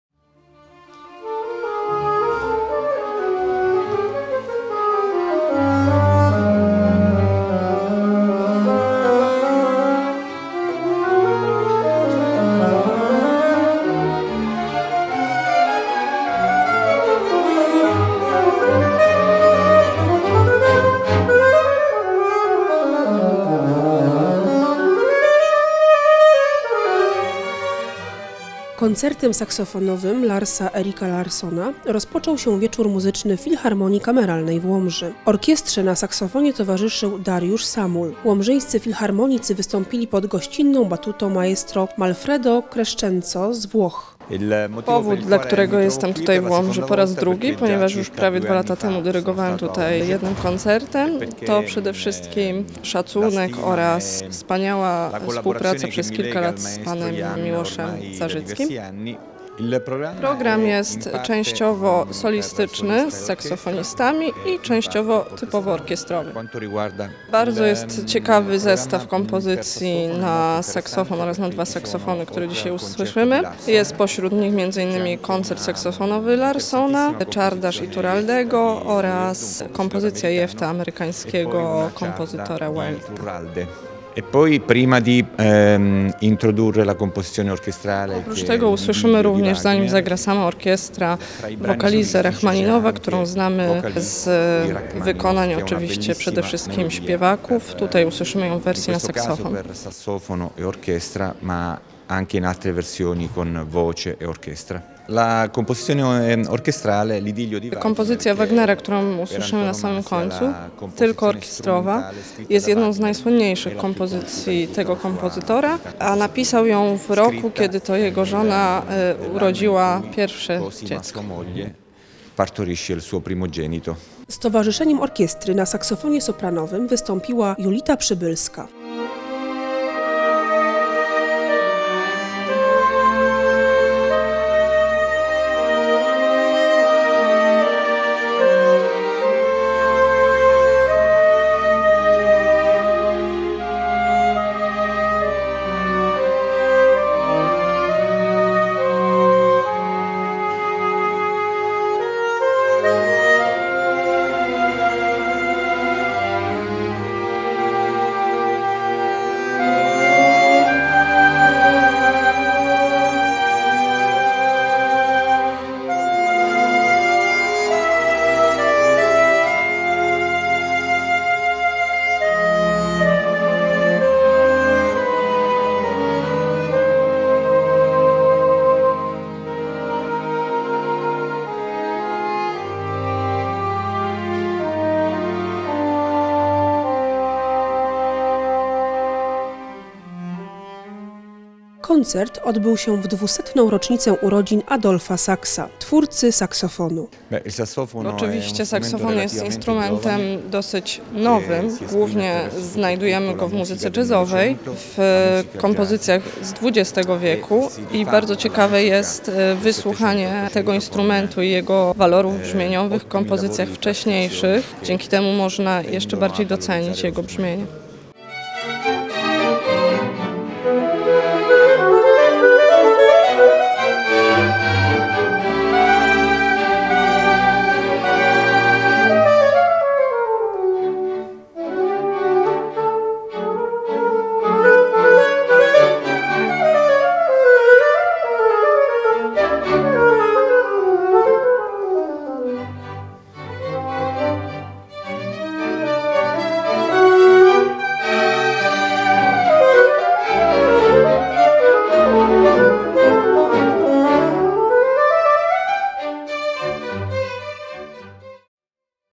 Filharmonia Kameralna w Łomży - "Saksofon w trzech odsłonach" cz.1
Łomżyńscy filharmonicy wystąpili pod batutą Manfredo di Crescenzo - dyrygenta z Włoch...